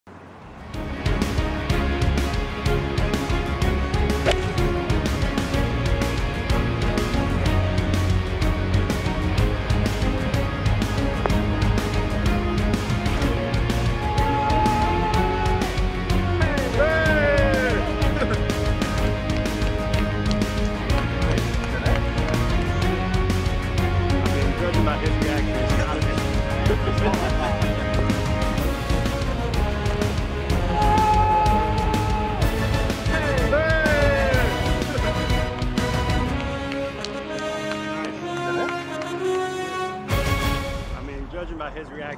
Byeong Hun An’s Amazing Hole-in-One from 189 yards | 2020 PGA Championship
An ace from Byeong Hun An in the final round of the 2020 PGA Championship. He took exact aim on the 189-yard 11th hole at TPC Harding Park and made it count, drawing cheers from volunteers and competitors alike.

for all the details.